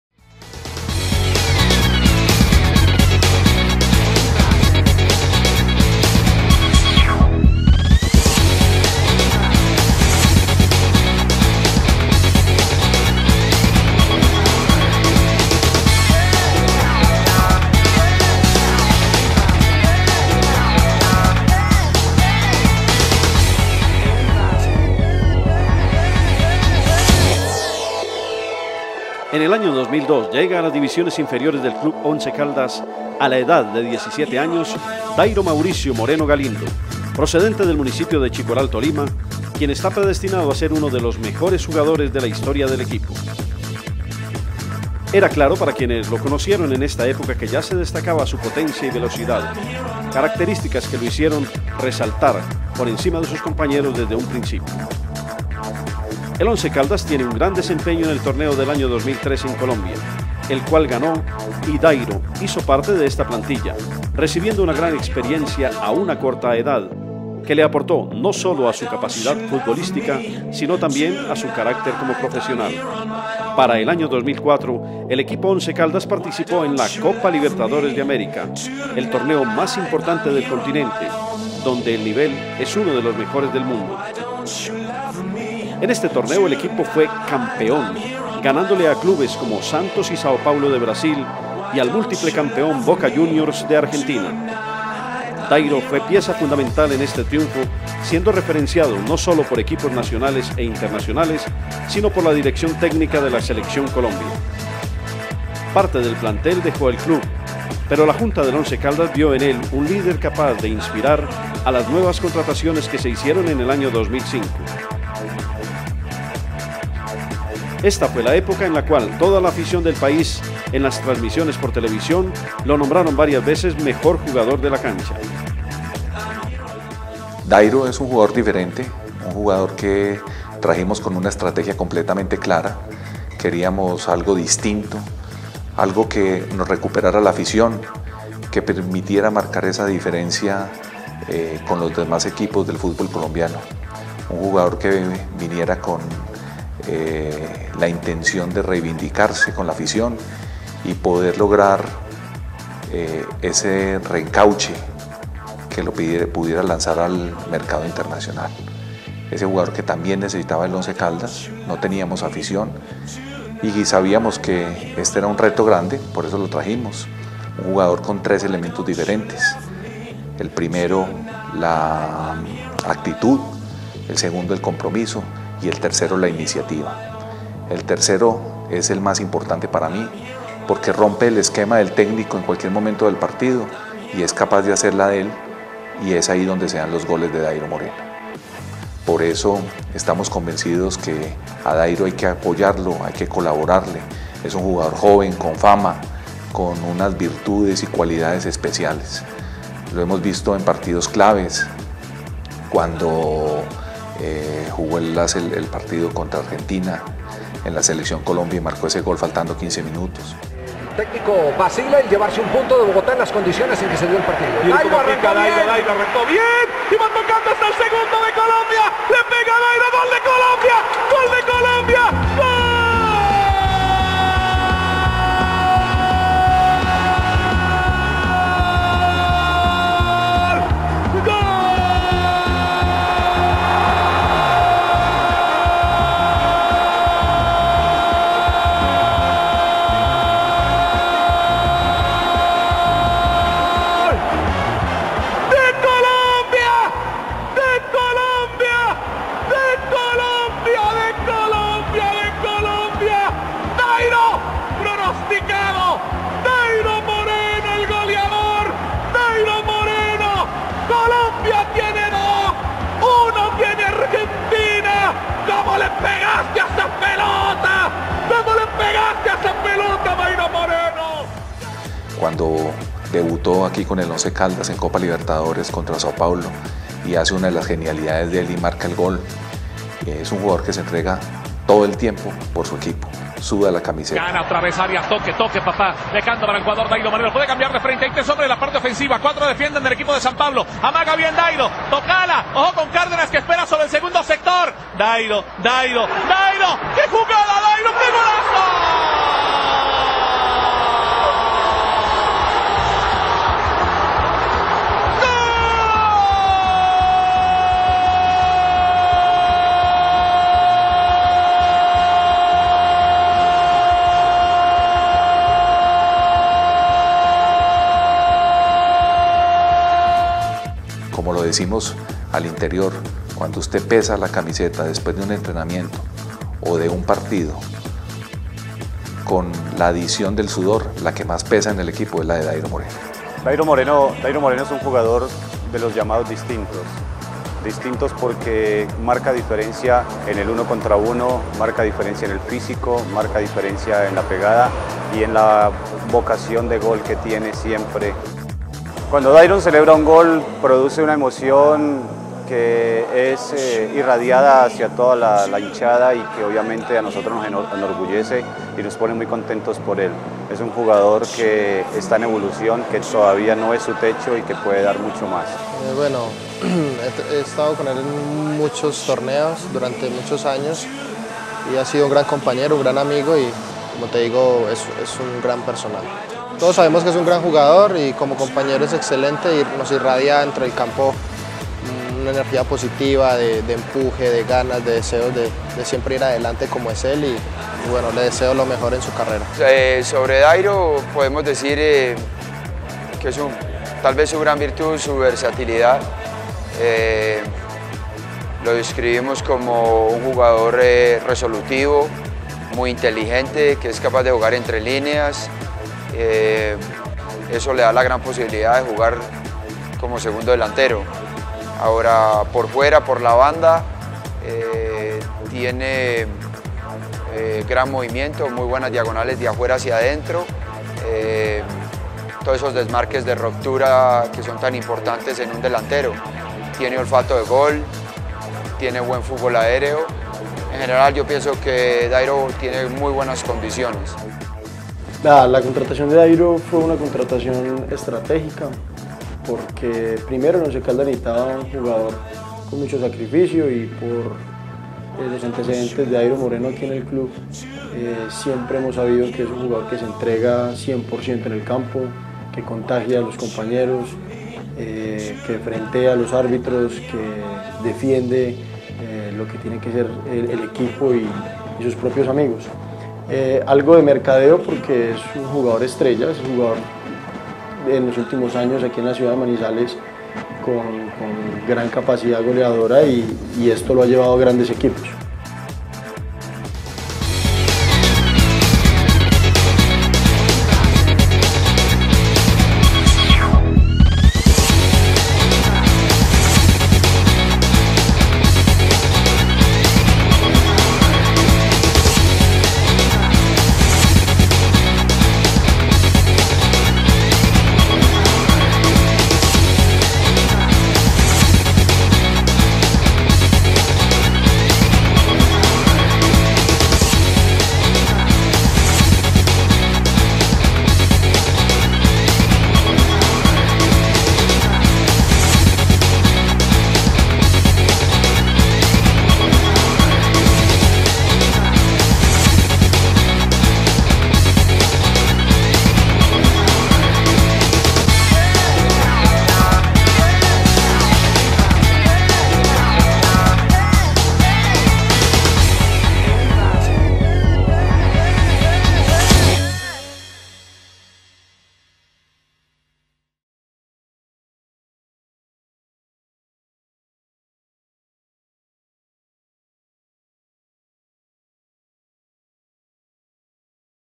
Dayro Moreno - Colombian Soccer Player for Once Caldes
Dayro Mauricio Moreno is a Colombian Footballer (soccer) player from Chicoral, Tolima, Colombia.  Moreno is currently a Striker for the Colombia's Category A team; Once Caldas and is represented by FIFA agent Alvaro Munoz Castro of Munoz Castro International Soccer.